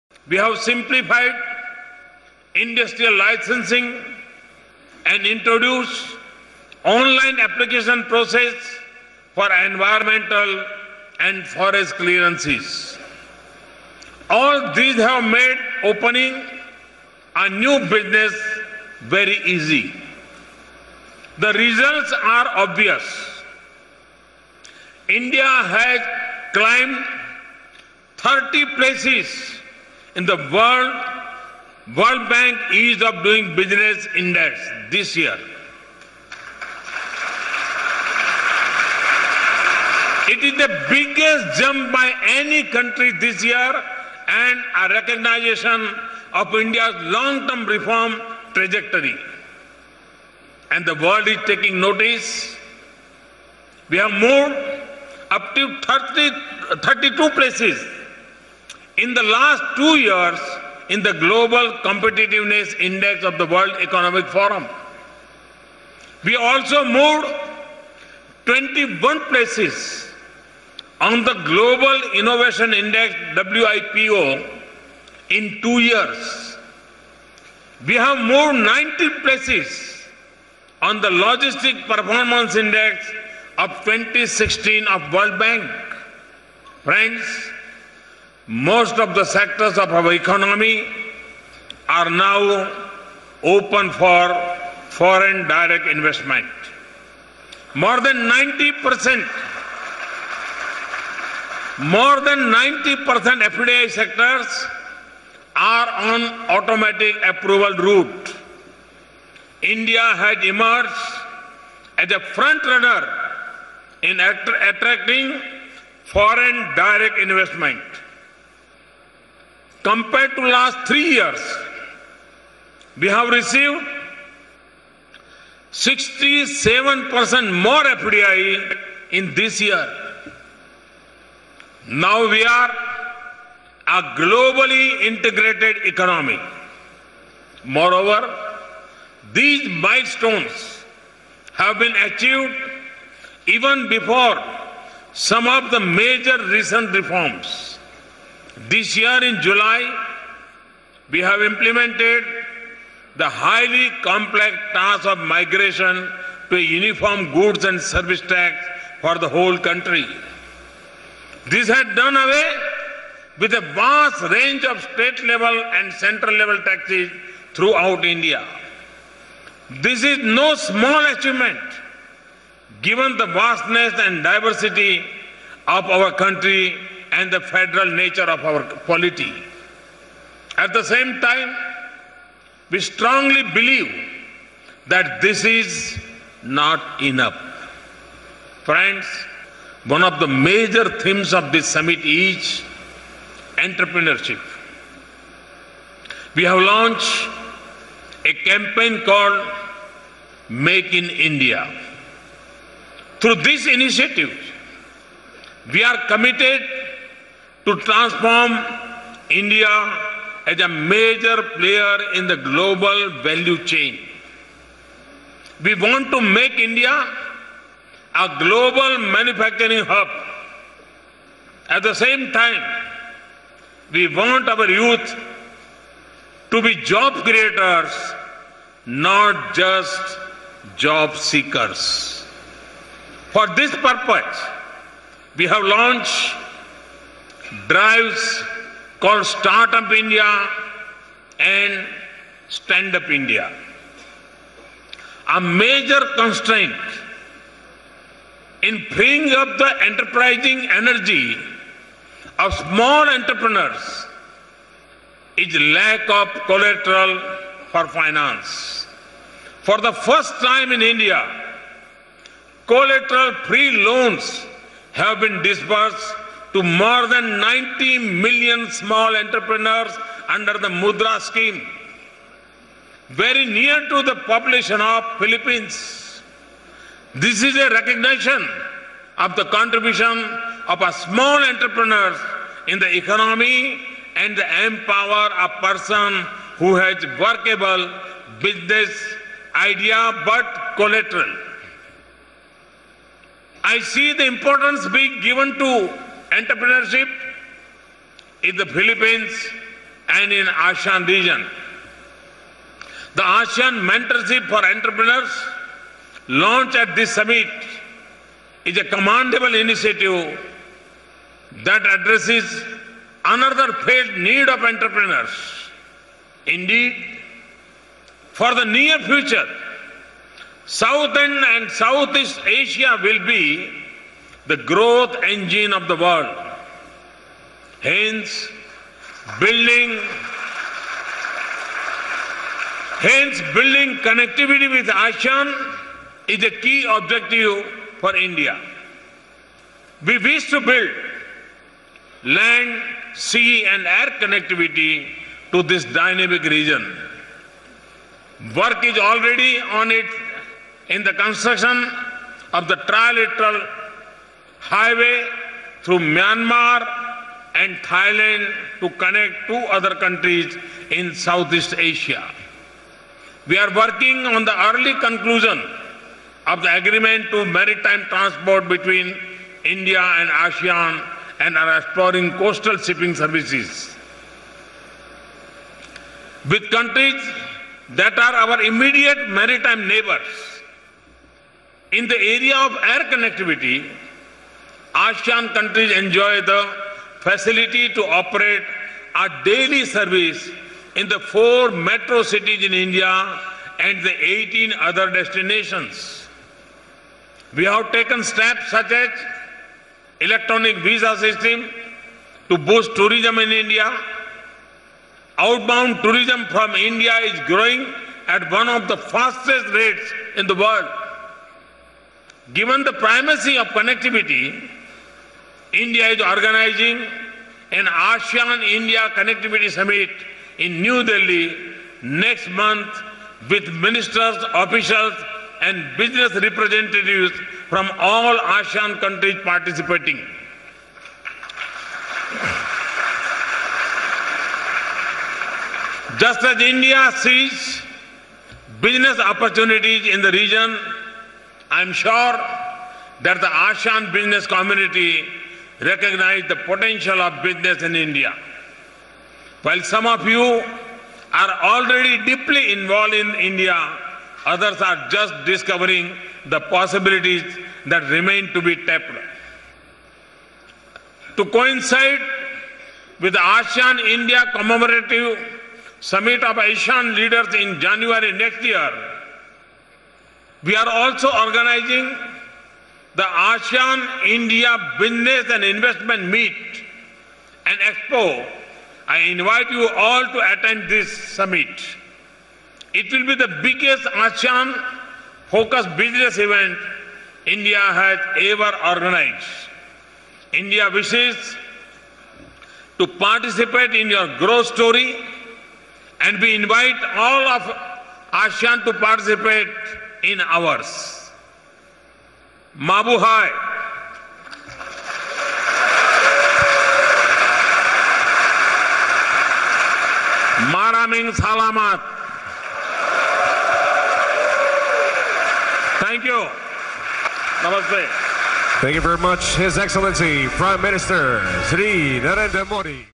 आसान, असरदार और पारदर्शी शासन देने के लिए सरकार दिन-रात काम कर रही है: PM मोदी
प्रधानमंत्री नरेंद्र मोदी ने कहा है कि भारत को पूरी तरह बदलने का काम अभूतपूर्व स्तर पर चल रहा है। मनीला में आसियान सम्मेलन को संबोधित करते हुए मोदी ने कहा कि आसान, असरदार और पारदर्शी शासन देने के लिए सरकार दिन-रात काम कर रही है।